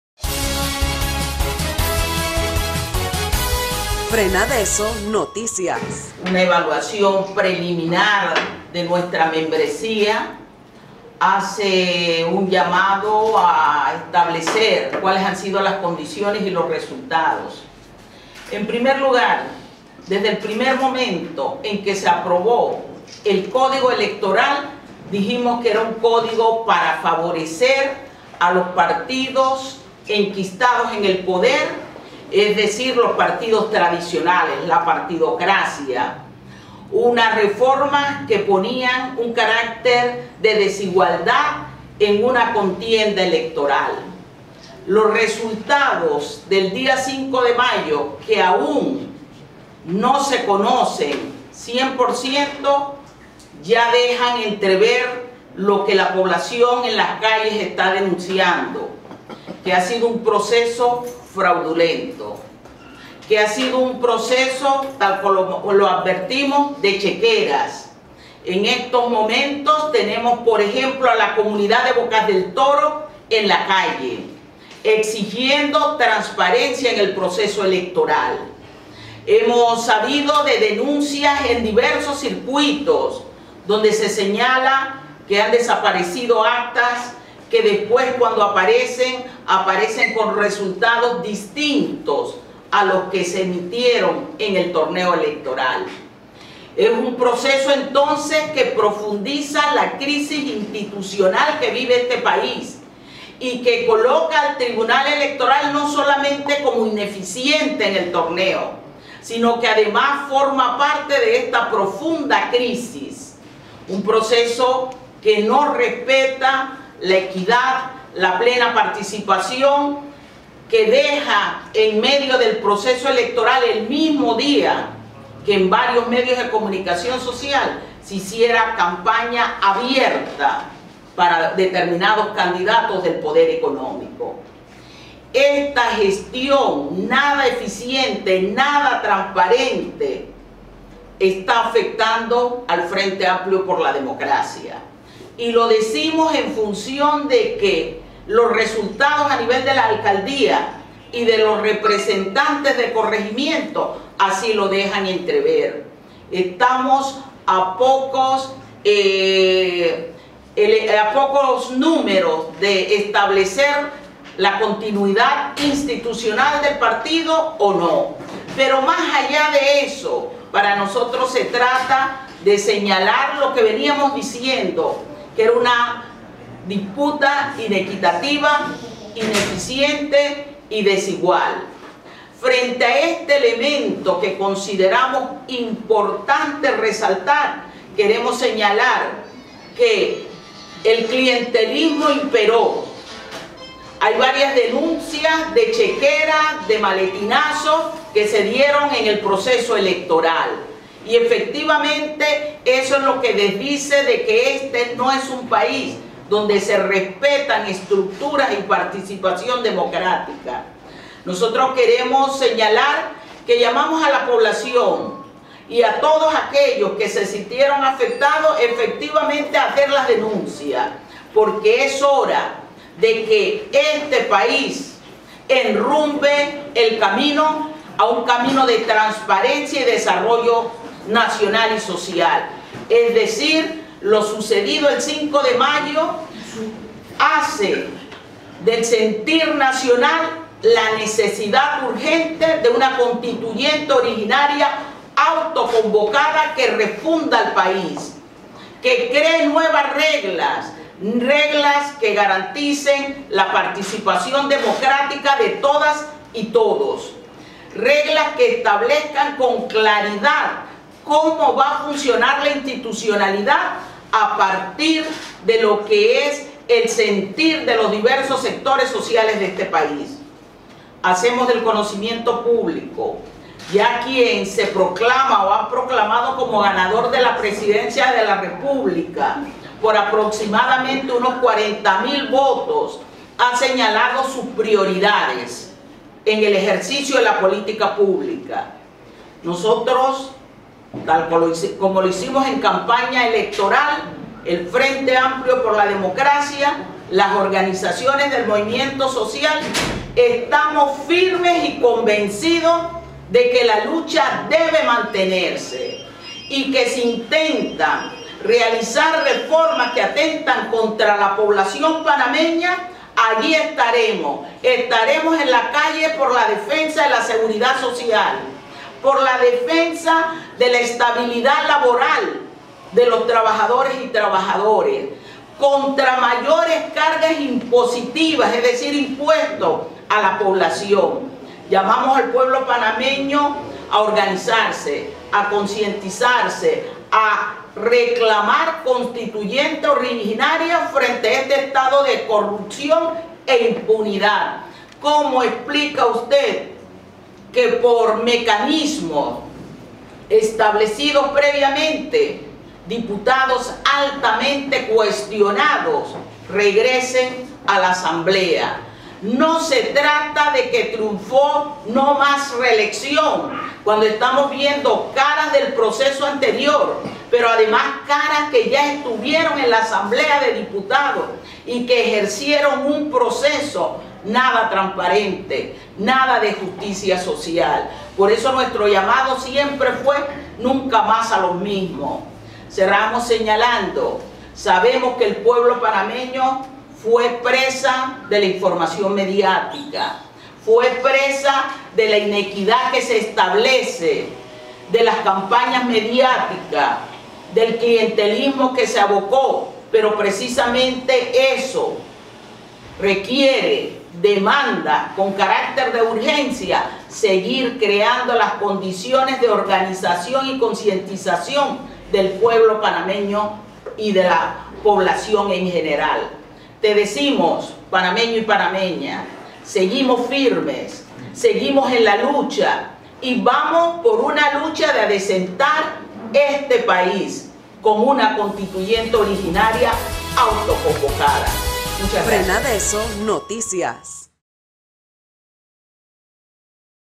Conferencia de prensa FAD, denuncia proceso electoral fraudulento
FAD denuncia proceso electoral fraudulento

Este ha sido un proceso electoral amañado y fraudulento desde la aprobación de reformas electorales antidemocráticas que denunciamos, la proliferación de encuestas para manipular a la opinión pública, la campaña de odio y miedo a través de los medios de comunicación contra el FAD y sus candidatos y el clientelismo vulgar incluso el mismo día de las elecciones.

Este proceso fraudulento, con pérdida de actas, compra de votos, inconsistencias, irregularidades, afecta los números para la subsistencia del partido que apenas por una décima estamos a punto de alcanzar.

Llamamos a la población que se siente burlada en su voluntad popular a realizar las protestas y denuncias necesarias.
Llamamos al pueblo a prestar atención a las prioridades expresadas por el candidato presidencial que afectan los intereses de los sectores populares.

Llamamos al pueblo a seguir la lucha por una Asamblea Constituyente Originaria con plenos poderes, ni reformas ni parches que aprueben una Asamblea que seguirá siendo corrupta.
La crisis no se resuelve con los resultados de estas elecciones, por el contrario se ahonda.

Llamamos al pueblo a la organización y lucha hasta alcanzar democracia participativa y plena.

El FAD anunciará los pasos a seguir luego de contar con los resultados finales y luego del indispensable balance a nivel nacional con nuestras estructuras, aliados y candidatos.

Panamá, 8 de mayo de 2019.